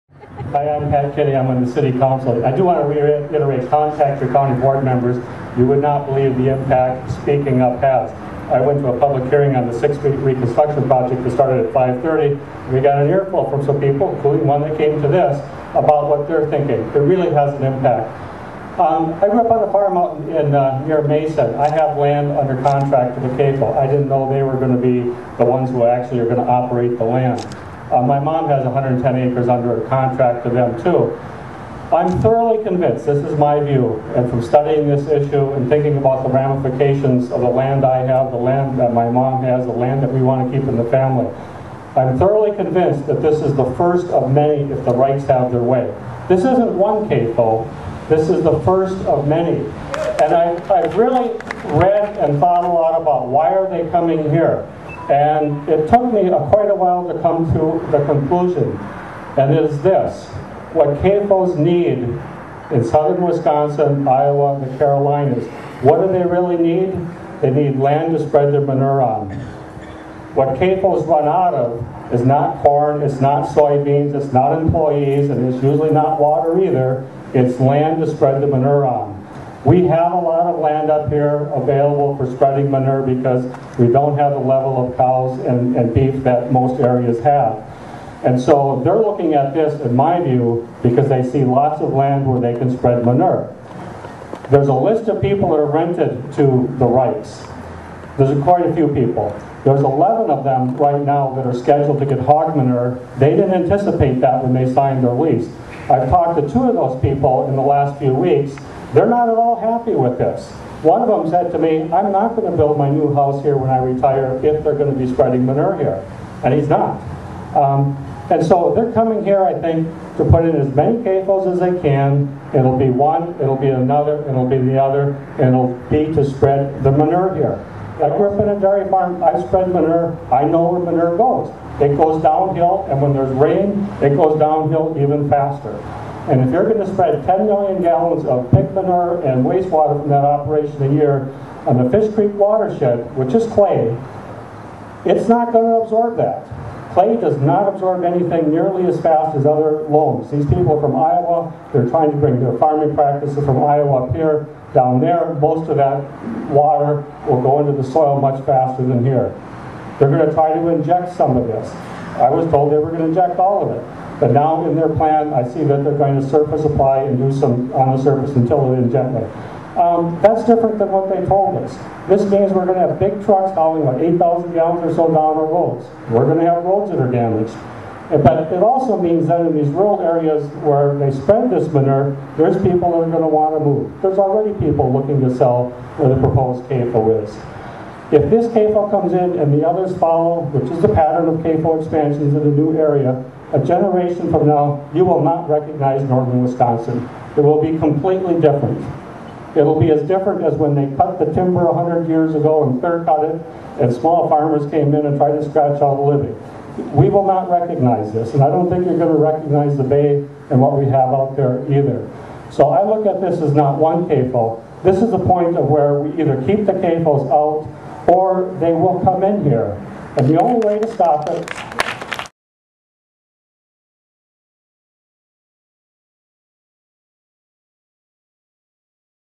Pat Kinney
Ashland City Councilor speaking at Stink-In in Ashland.